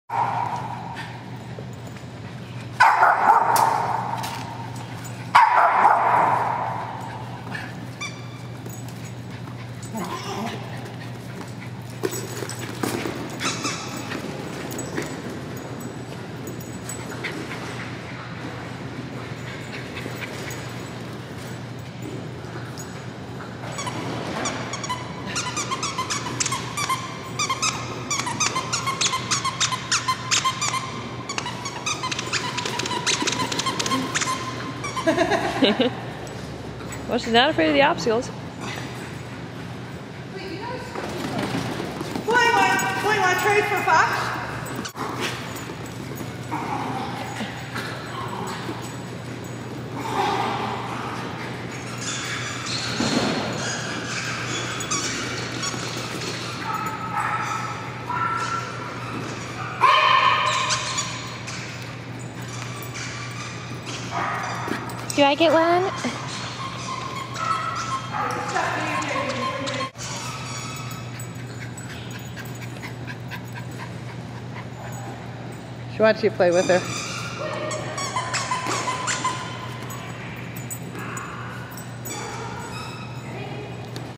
11 04 09 Toggle first time at K9
Toggle's first few minutes at K9.  This is her first 'agility lesson', all we want her to learn is that this place is fun, there's a lot of stuff to navigate around, and moving fast is important